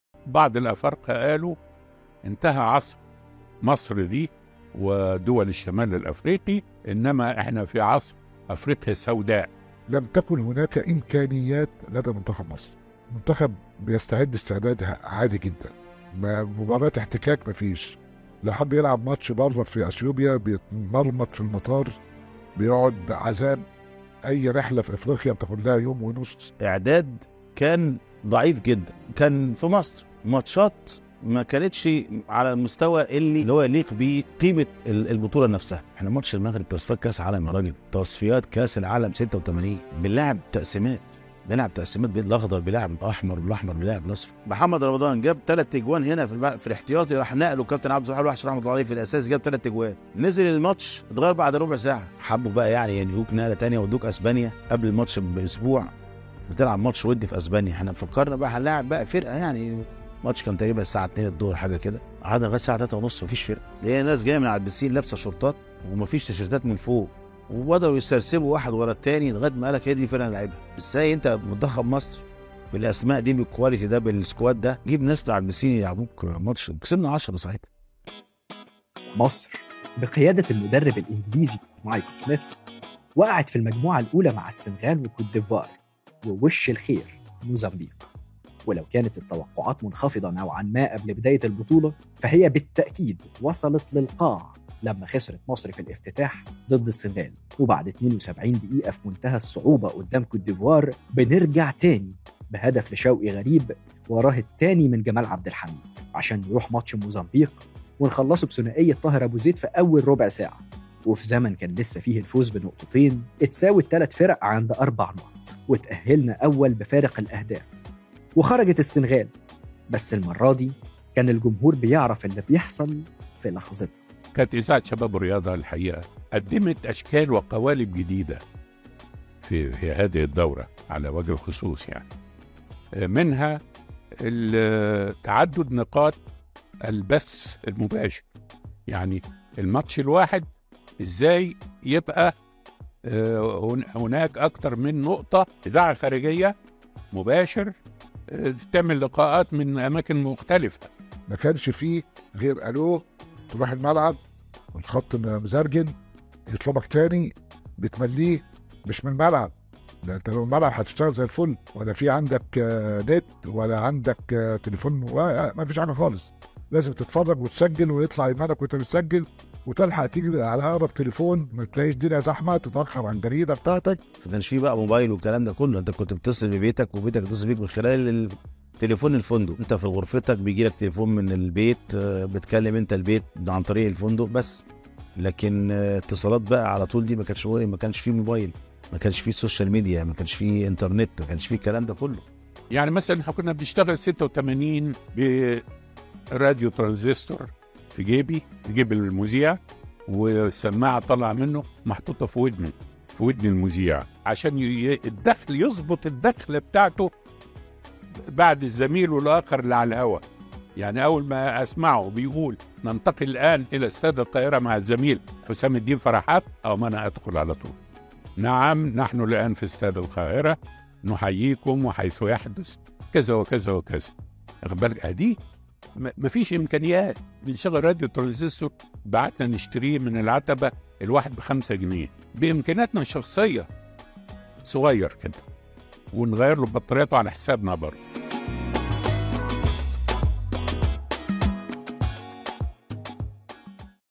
فيلم في الجول - كُتَّاب التاريخ واستعدادات مصر الصعبة في 86
فيلم في الجول - كُتَّاب التاريخ

🗣️ جابوا ناس من على البسين يلاعبوا منتخب مصر

توقعاتنا للفوز كانت في القاع قبل مواجهة وش السعد 💪🇪🇬